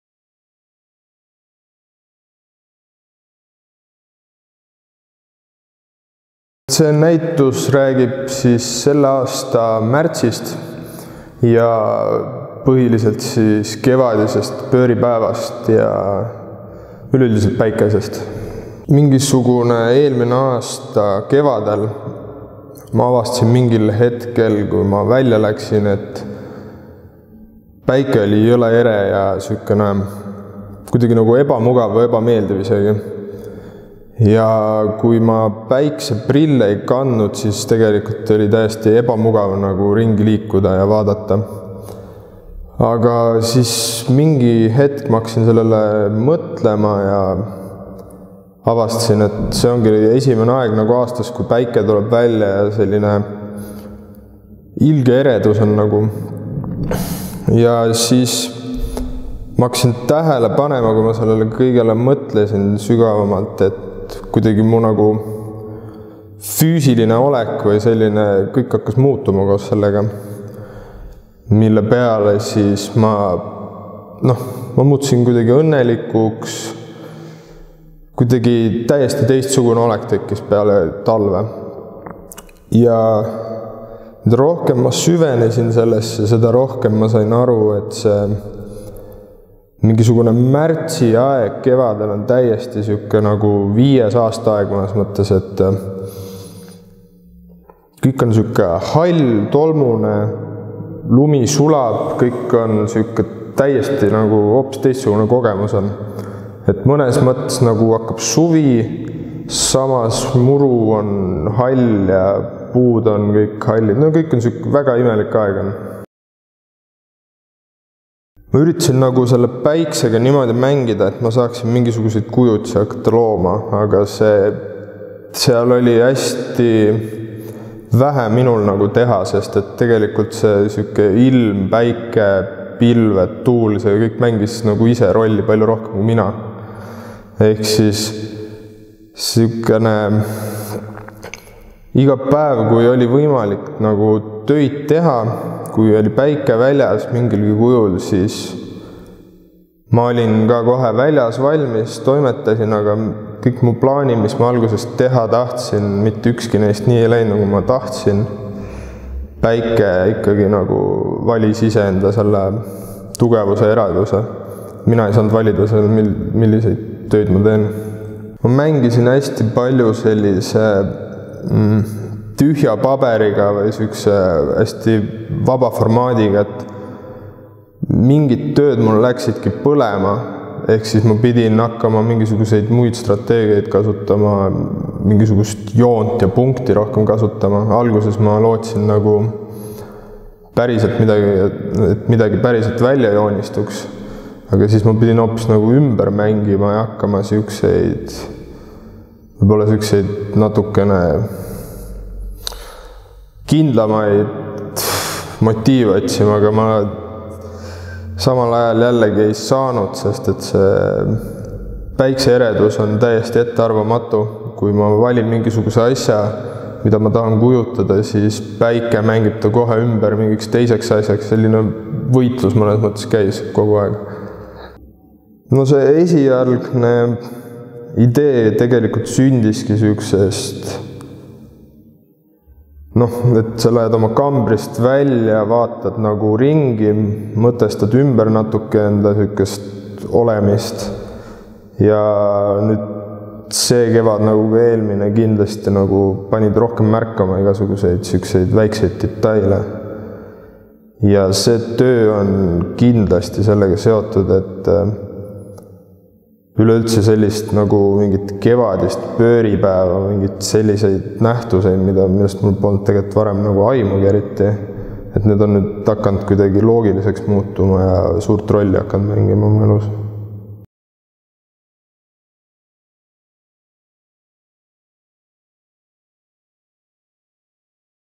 Siim Elmers "Seniit"
Siim Elmers räägib oma isikunäitusest "Seniit" Tartu Kunstimaja väikeses saalis 03.05.–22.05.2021.

Igal kevadel, kui lumi sulama hakkab ja ilm halliks pöörab, tean, et varsti tuleb päike. Märtsis ja aprillis on päike eriti ere ja kõrvetav. Sulanud lume ja põletava päikese koosmõjul tekib aeg, mil kogu maailm näib mattuvat tolmukihi alla. Jääb mulje nagu oleks maailm ära pleekinud.

Päike mõjutab kõike, valgus on halastamatu jõud. Kivid murenevad, puit pudeneb, teras korrodeerub, plastik nõrgeneb. Päikese teele jääb harva asju, millest ta jagu ei saa. Paber pole erand. Kasutades läätse, saavutasin tulemuse, mida otsisin. Kontsentreeritud kujul on päikesekiired nii kuumad, et põletavad. Mina kontrollin läätse, aga päike seab piirangud. Mõni päev on ere, teisel päeval pean punkti põletamiseks seisma õues tunde. Mõni nädal on pilvine, ei saagi töötada. Päike dikteerib mu tööd.

Siim Elmers (snd 1997) lõpetas eelmisel aastal Eesti Kunstiakadeemia installatsiooni ja skulptuuri osakonna. 2017. aastal pärjati teda Noore Skulptori Preemiaga (koos Johannes Luigega). Tegu on tema esimese isikunäitusega.

Siim Elmers talks about his solo exhibition "Zenith" in the small gallery of the Tartu Art House 03.05.–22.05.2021.

Each spring, when the snow starts to thaw and the weather turns grey, I know, that soon the sun will come. In March and April, it is especially bright and scorching. The molten snow and burning sun in unison create a time when the whole world appears to be buried under a layer of dust. It seems like everything has faded.

The sun influences everything and light has a merciless power. Stones crumble, wood chips, steel corrodes, plastic fails. Not much can stand up to the sun and paper is not an exception. Using a lens I achieved what I was aiming for: in their concentrated form, the rays of the sun are so hot that they burn. I control the lens but the sun sets its own limitations. Some days are bright, but on others I have to stand outside for hours to burn a dot. Some weeks are cloudy and I cannot work at all. The sun dictates my work.

Siim Elmers (b 1997) graduated last year from the Department of Installation and Sculpture of the Estonian Academy of Arts. In 2017, he was awarded the grand prix of the Young Sculptor Award (with Johannes Luik). This is his first solo exhibition.